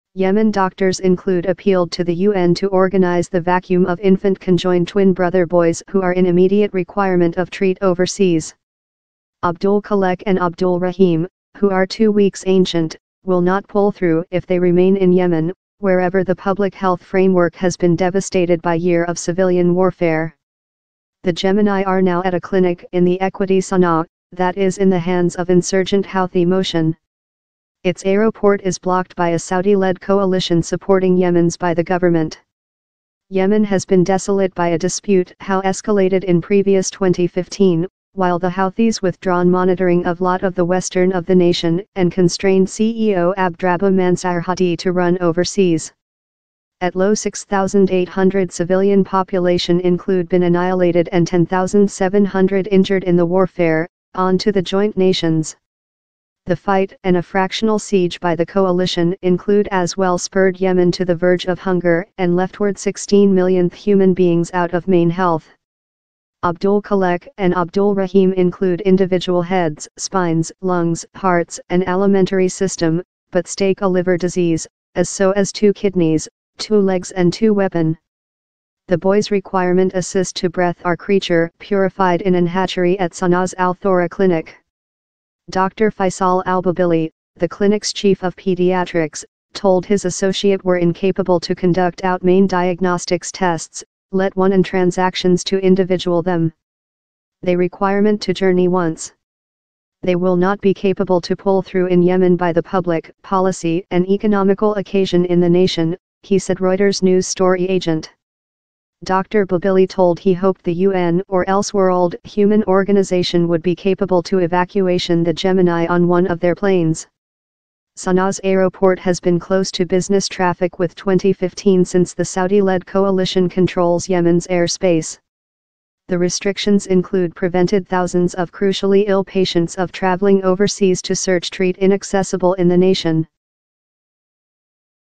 Yemen Conjoined Gemini: Doctors Circulation For Assist Evacuating Boys
The newborns need treatment abroad, but commercial planes cannot leave Sanaa because of a blockade.